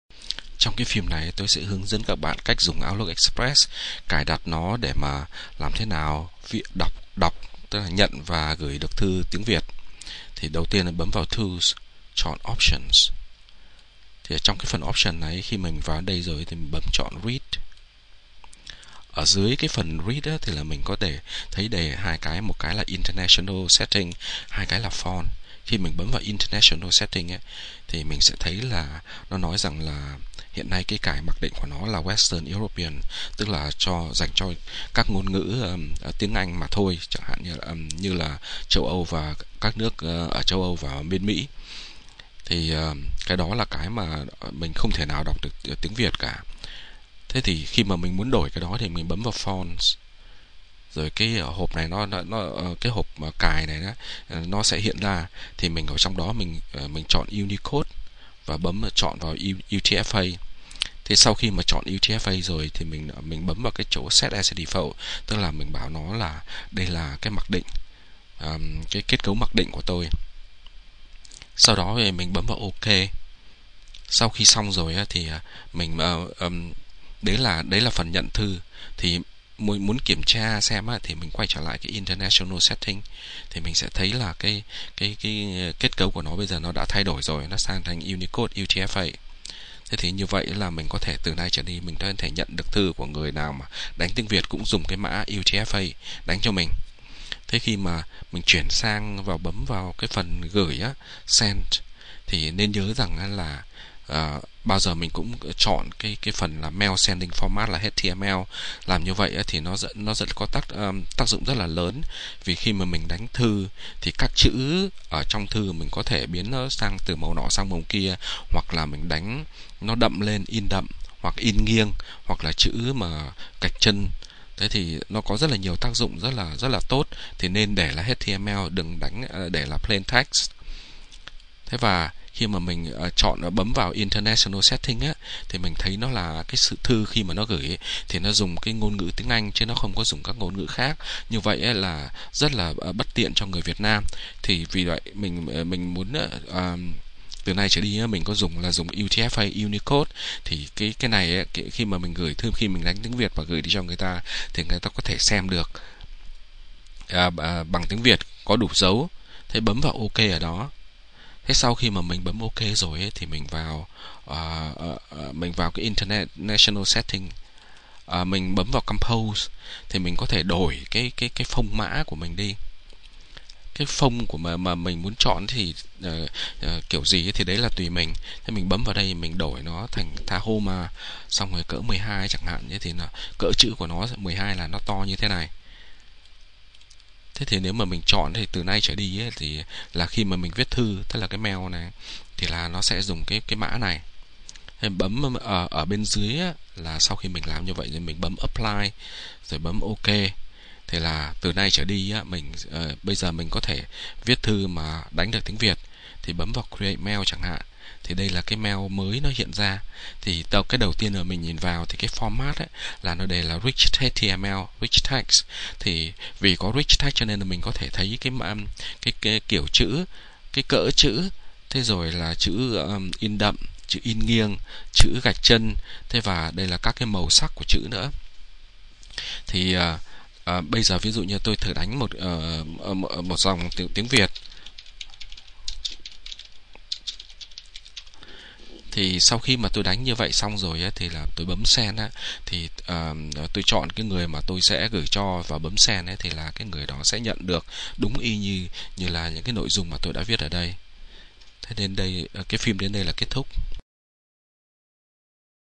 Windows XP - setup Outlook Express to send and receive Vietnamese e-mails
Phim này giới thiệu cách cài đặt chương trình phần mềm Outlook Express để gửi và nhận thư bằng tiếng Việt có dấu.